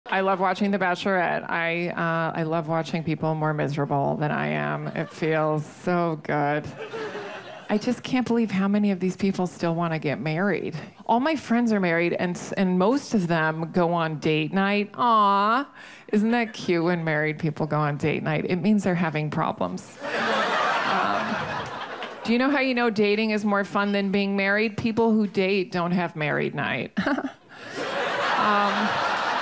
"I love watching 'The Bachelorette.'" 🎤: Carmen Lynch
Why Carmen Lynch loves The Bachelorette.